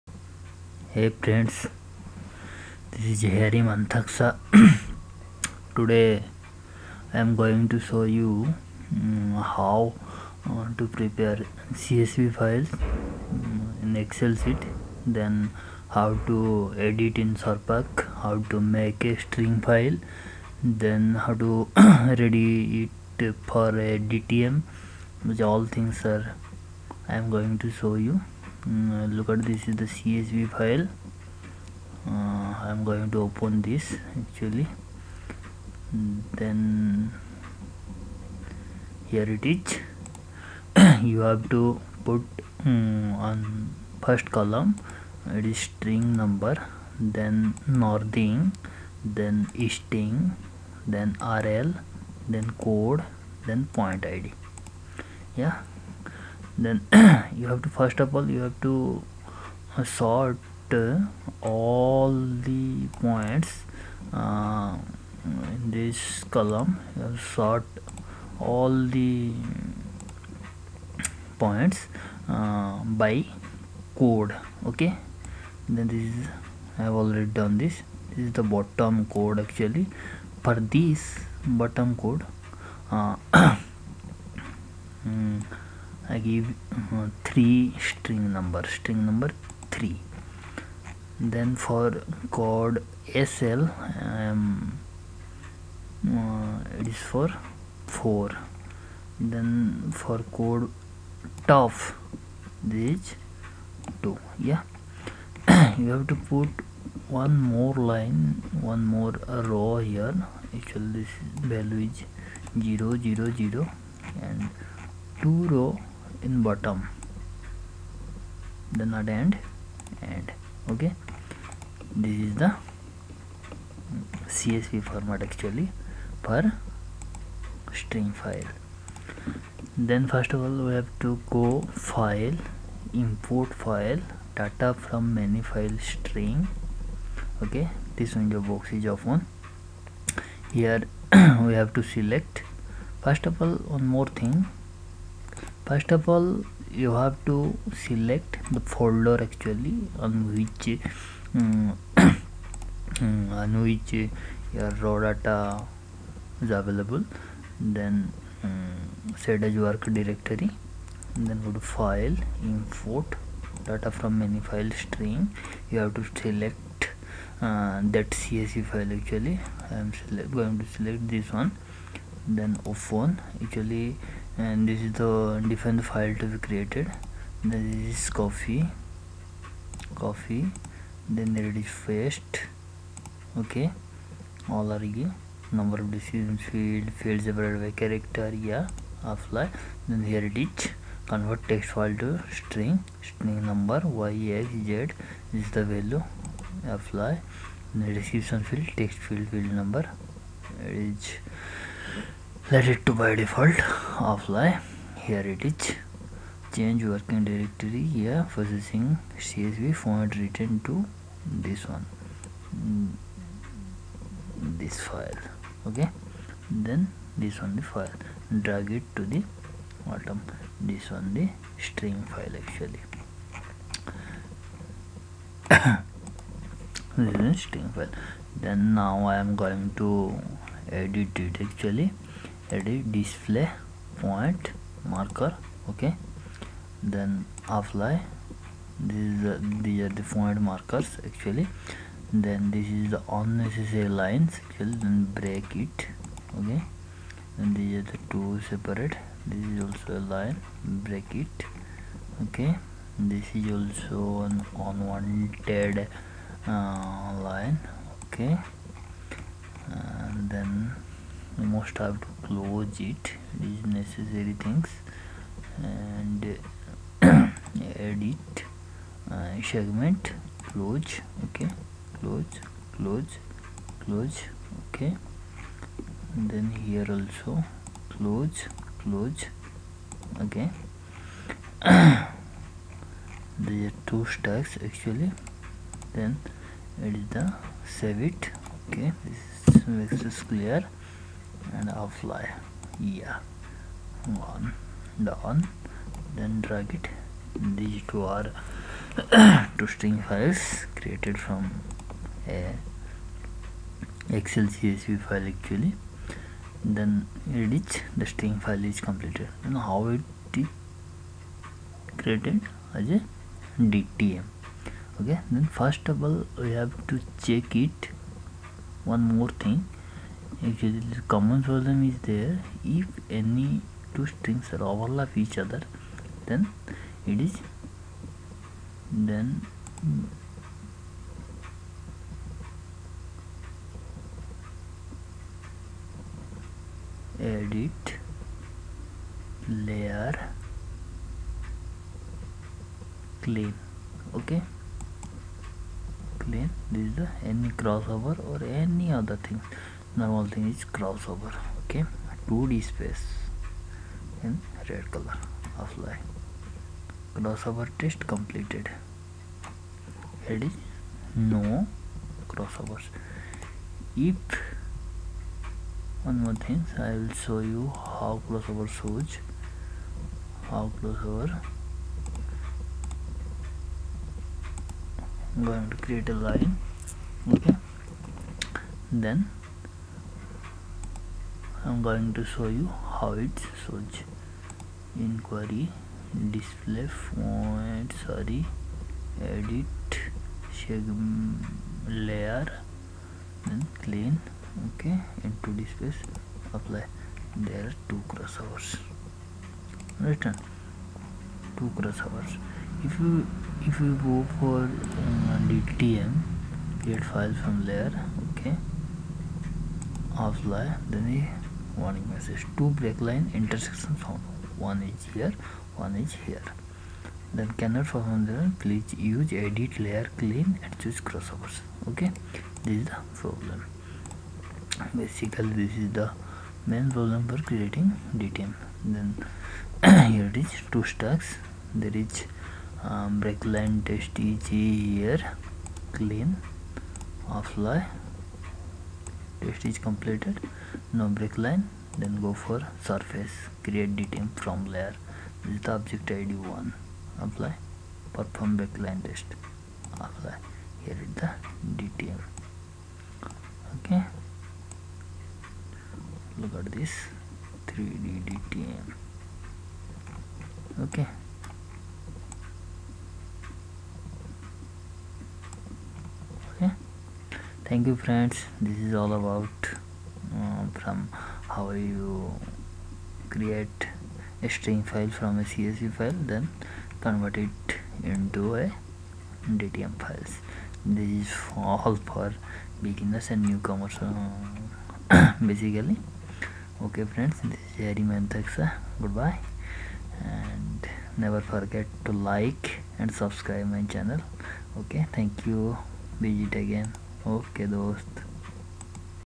SURPAC Tutorial - How to make string and DTM file in SURPAC
Hey Friends It is a video for beginners of SURPAC to learn how to make string, DTM file. Hope this video help you.